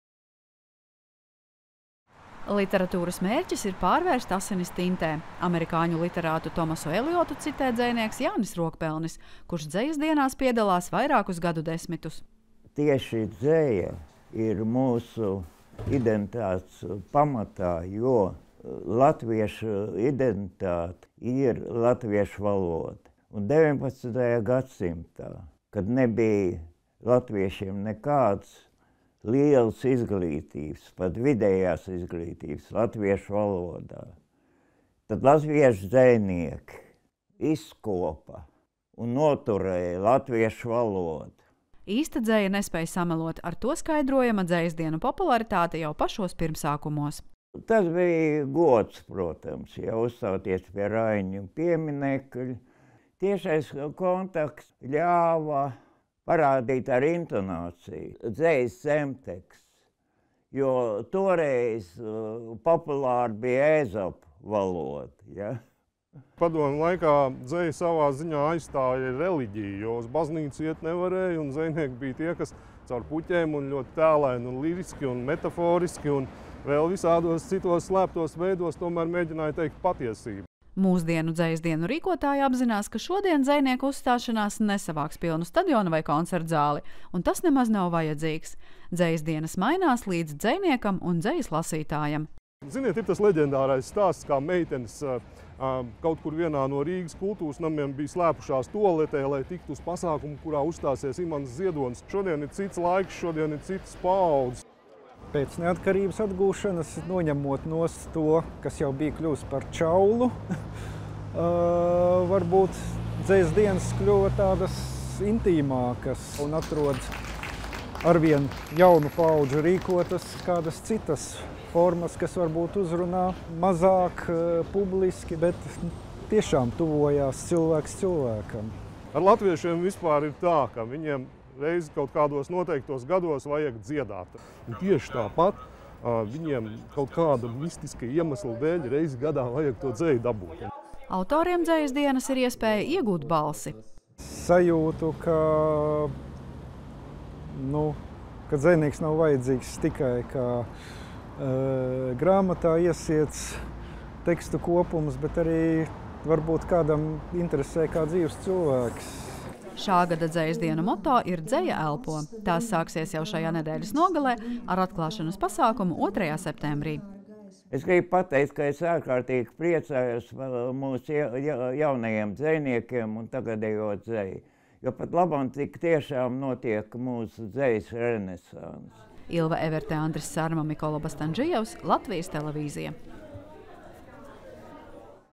Dzejas dienas šoruden – jau 58. reizi
Jau šīs nedēļas beigās tiks atklātas tradicionālās Dzejas dienas. Pirmoreiz mūsdienu izpratnē tās notika 1966. gadā, un tas nozīmē, ka pēc diviem gadiem Dzejas dienu pasākumi tiks rīkoti jau 60 reizi. Kāpēc ikgadējiem dzejas lasījumiem un autoru sarunām ar savu auditoriju ir tik ilgs mūžs? Kolēģe Ilva Everte to jautāja Dzejas dienu rīkotājiem un dalībniekiem.